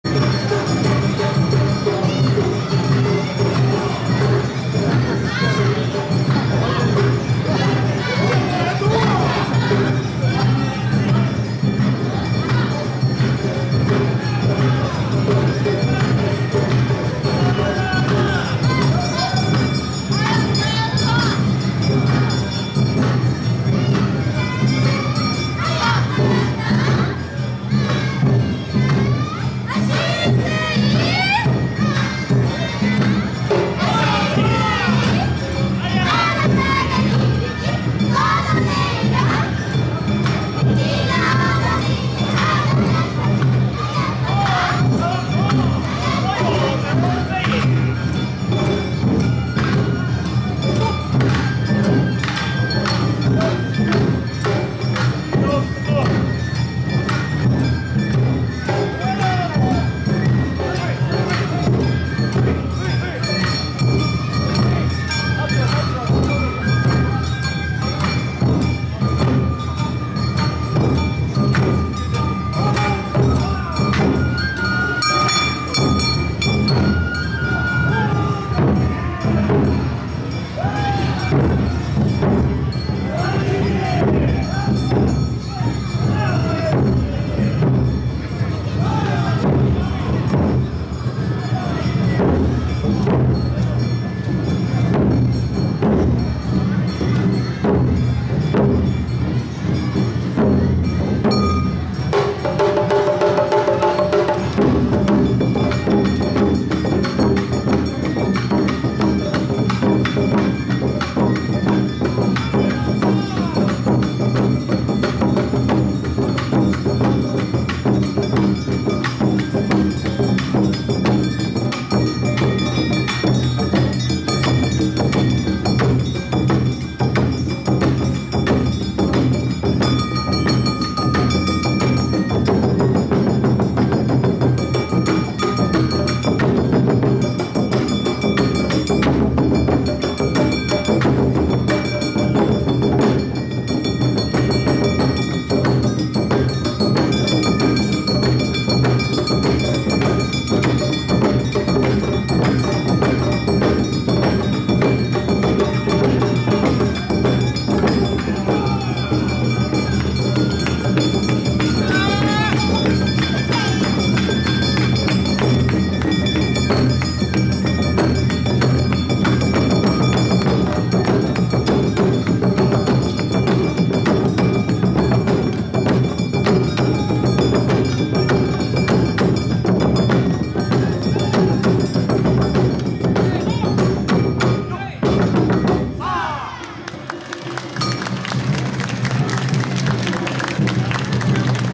新粋連／流し踊り第二会場／第54回 中目黒夏まつり 阿波おどり2019
2019/08/03(令和元年8月3日）
第54回 中目黒夏まつり～阿波おどり～2019
【場所】東横線・東京メトロ日比谷線中目黒駅周辺一帯
【主催】中目黒駅西銀座商店街振興組合
【後援】目黒区、中目黒商店街連合会
【協力】中目黒GT